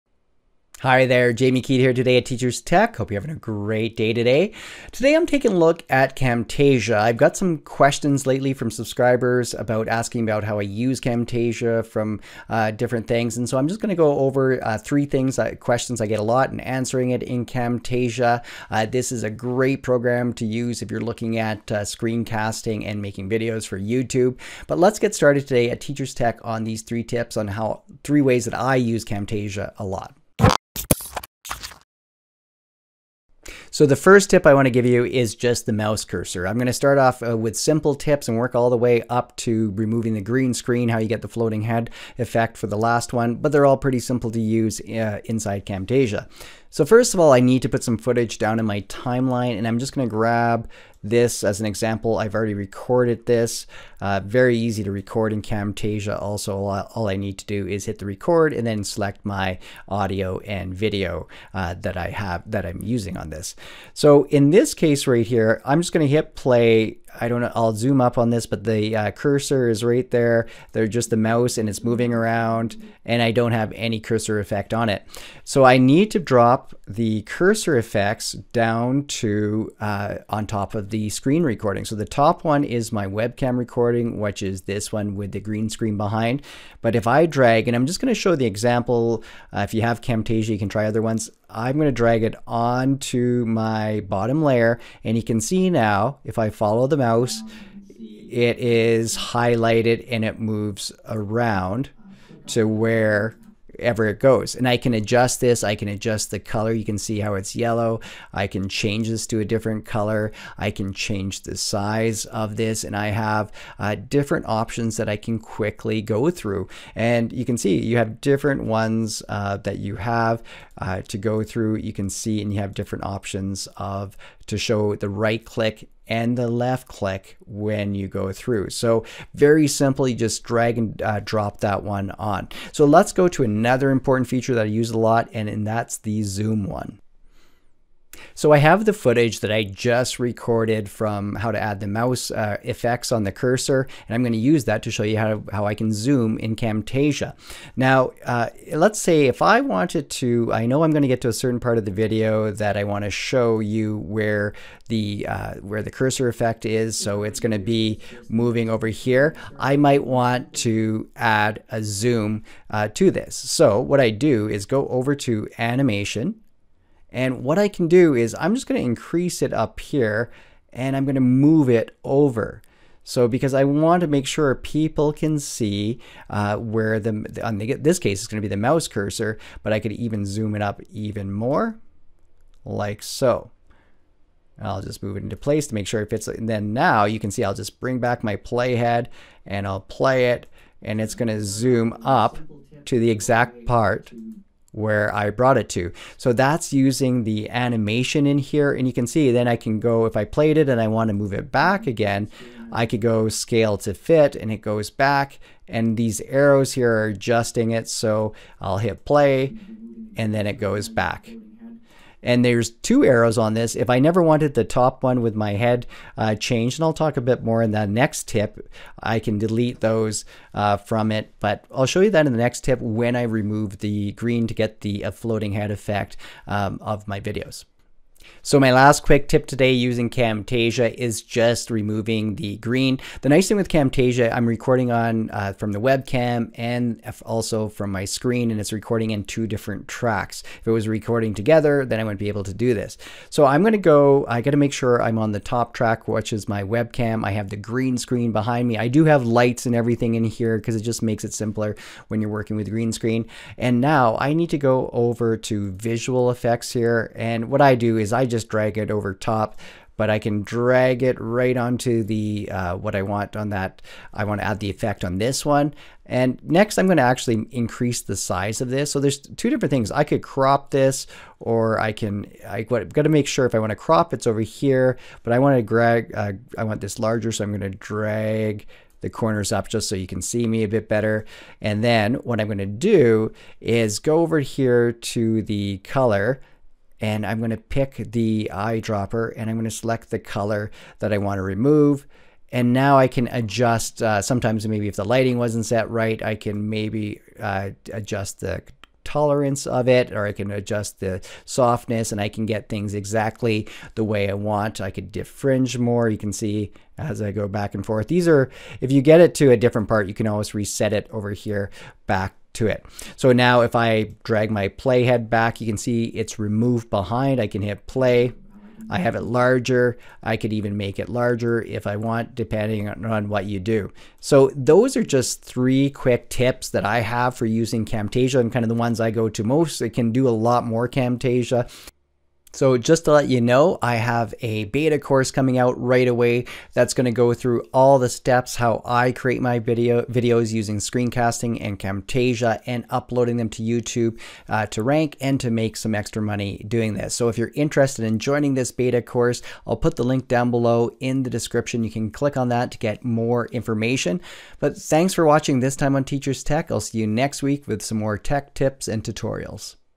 3 Ways I use Camtasia to Make my Videos
Let me show you how I use Camtasia 2019. Camtasia creates professional-looking screencasts in the easiest possible way. Theses 3 features that I use most often to make my screencast in Camtasia 2019.